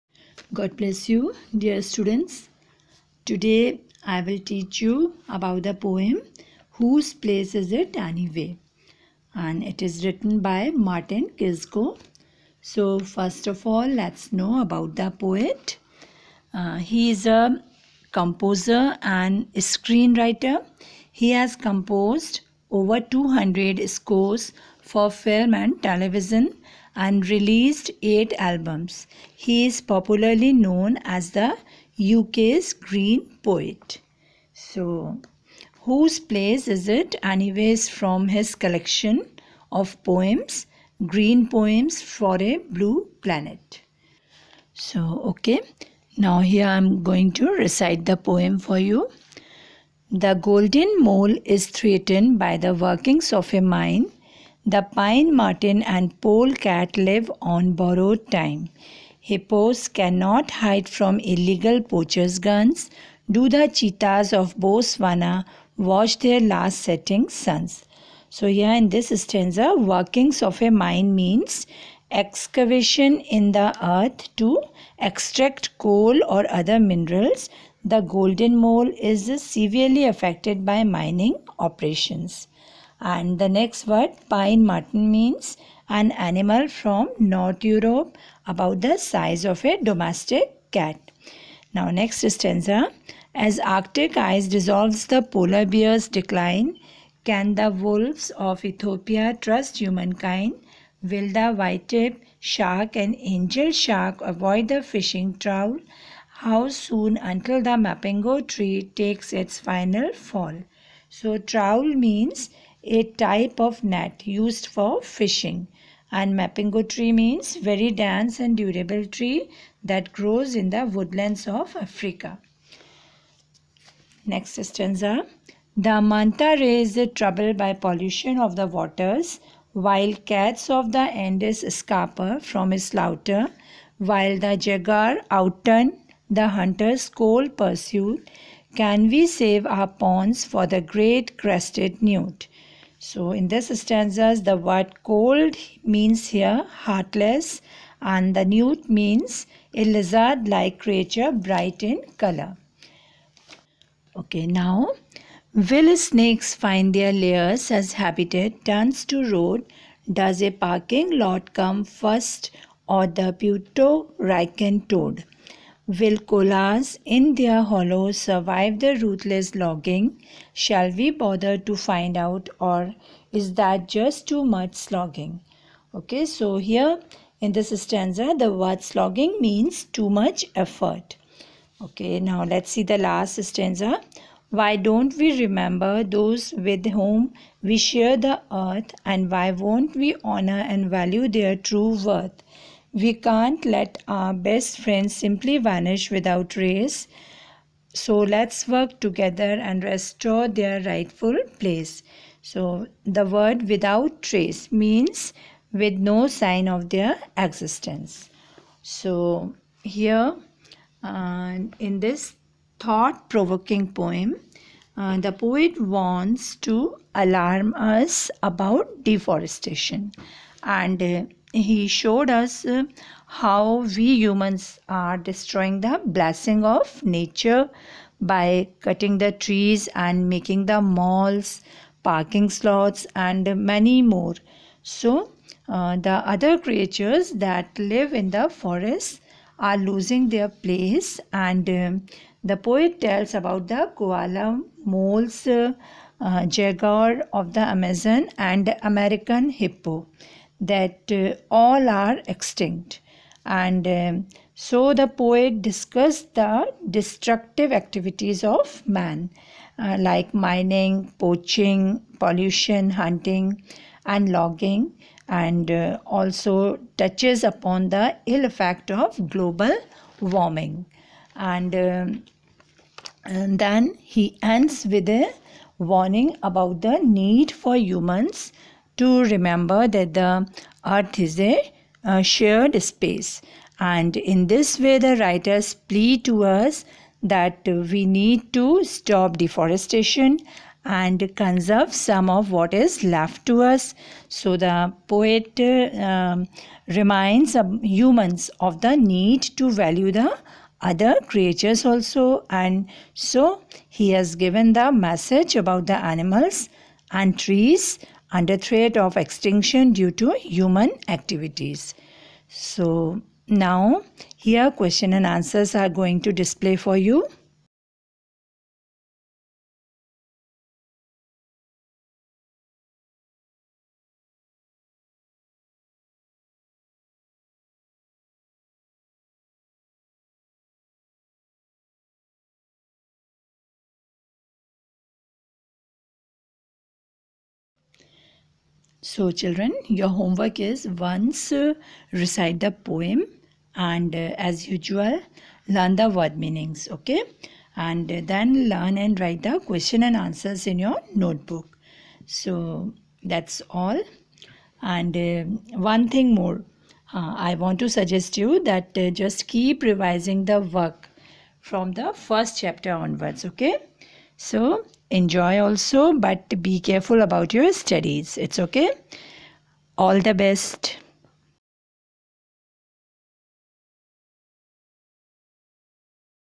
English Literature| Class 8| Poem| "WHOSE PLACE IS IT ANYWAY?"
Summary of poem and questions and answers.